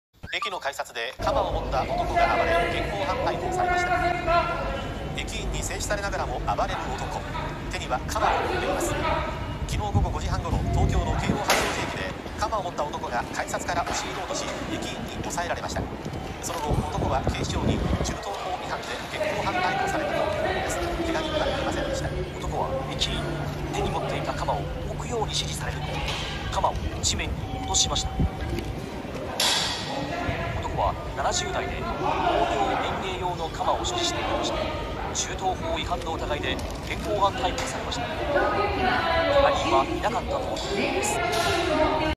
#574【危険騒然】京王線の京王八王子駅、鎌所持の男は警視庁に銃刀法違反で現行犯逮捕、駅員が押収して刺股(さすまた)で制止。目撃者が撮影した一部始終映像。京王線刺傷事件の影響で緊張した状況
【危険騒然】京王線の京王八王子駅、鎌所持の男は警視庁に銃刀法違反で現行犯逮捕、駅員が押収して刺股(さすまた)で制止。目撃者が撮影した一部始終映像。京王線刺傷事件の影響で緊張した状況。
再生速度は視聴者自分で0.5倍と0.75倍に調整してください、見てくれてありがとう。

目擊者『京王八王子駅に、鎌を持ったおじさん「鎌じい」出現』。
7月10日午後5時半ごろ、東京の京王八王子駅。赤い服を着た男が、駅員に刺股(さすまた)で改札の外へ押し出されています。よく見ると、男の右手には、鎌があります。男は刺股をつかみ、抵抗。その後、駅員を押し返す形で改札の中に…。駅員も後ずさりしながら、必死に抵抗。すると、男は盾を持った女性駅員のほうへ近付いていきます。その時…。
駅員「危ないから！危ないから置いて！下に置いて下さい！」。
男は駅員に、手に持っていた鎌を置くように、指示されると、鎌を地面に落としました。男は70代で、農業園芸用の鎌を所持していたとして、銃刀法違反の疑いで現行犯逮捕されました。けが人はいなかったということです。動機については、明らかになっていません。けが人はいませんでした。

京王線刺傷事件(けいおうせん ししょうじけん)は、2021年(令和3年)10月31日に東京都調布市を走行中の京王電鉄京王線特急列車車内で発生した殺人未遂事件である。

ニュース・ソース(ニュースの取材源) / News source
まりんぬ @marinnudesu
京王グループ KEIO